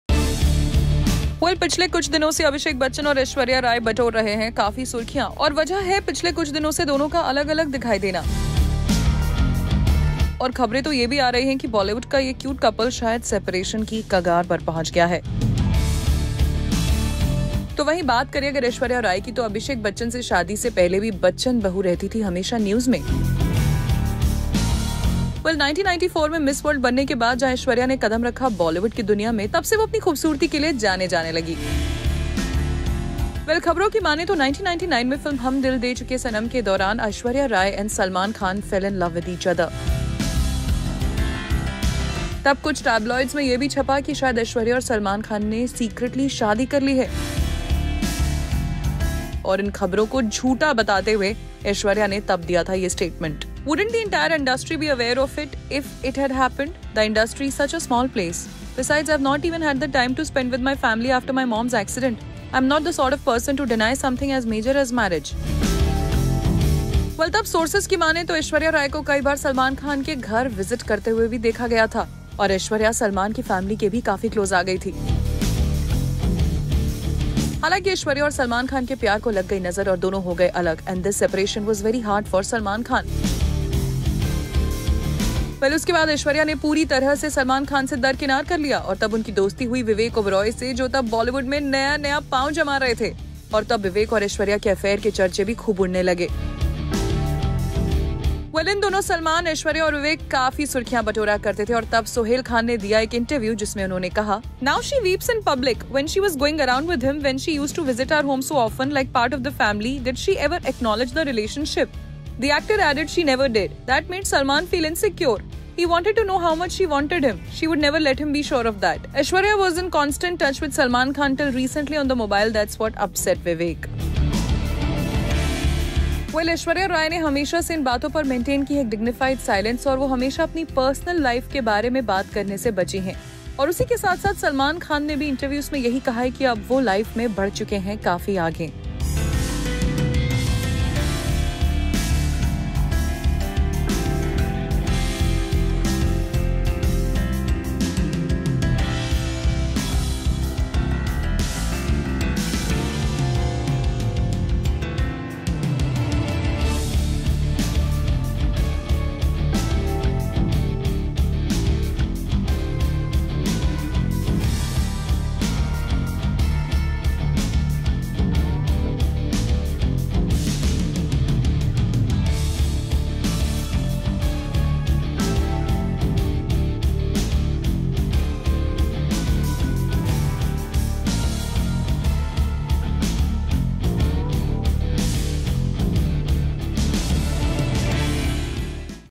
जब Sohail Khan ने Aishwarya Rai पर दिया था एक चौंकाने वाला बयान! | Salman Khan | Hindi News
90 के दशक में ऐश्वर्या राय और सलमान खान का रिश्ता 2001 में ख़त्म हो गया, जिससे प्रशंसकों को झटका लगा। ऐश्वर्या ने बाद में खुलासा किया कि उन्हें अपने रिश्ते के दौरान मौखिक, शारीरिक और भावनात्मक शोषण का सामना करना पड़ा, जिसके कारण उनका अलगाव हुआ. उन्होंने सलमान की बेवफाई और शराब के कारण गलत व्यवहार का जिक्र किया. सलमान के भाई सोहेल खान ने प्रतिक्रिया देते हुए अपने रिश्ते को सार्वजनिक रूप से स्वीकार नहीं करने के लिए ऐश्वर्या की आलोचना की. उन्होंने दावा किया कि खान परिवार द्वारा परिवार जैसा व्यवहार किए जाने के बावजूद उन्होंने सलमान को असुरक्षित महसूस कराया.